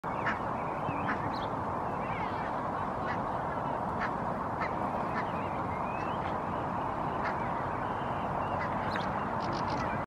Barnacle Geese in a spring evening (take #1)
Barnacle Geese (Branta leucopsis) in a spring evening; grazing in the park, vocalizing.

Valkoposkihanhia (Branta leucopsis) kevätiltana; laiduntamista puistossa, ääntelyä.